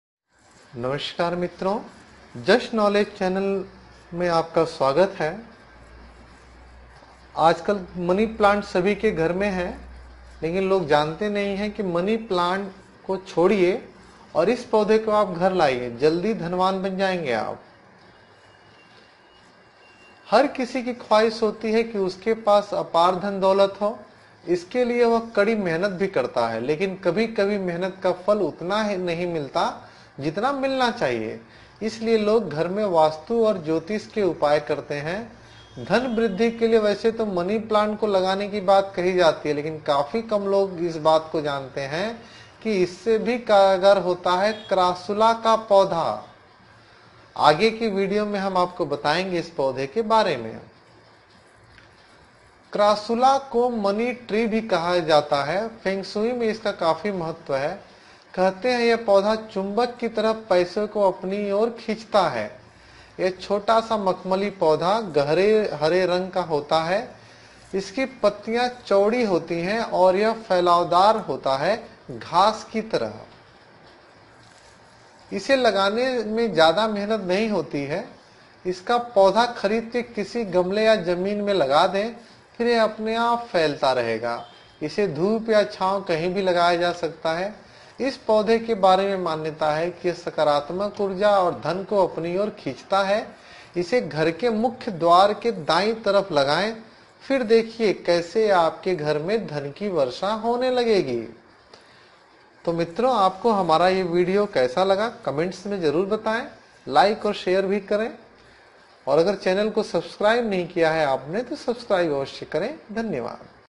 Instead of Money plant, plant this for money, मनी प्लांट छोड़िए और इस पौधे को घर लाइए, बनेंगे धनवान
Instead of Money plant, plant this for money, मनी प्लांट छोड़िए और इस पौधे को घर लाइए, जल्दी बनेंगे धनवान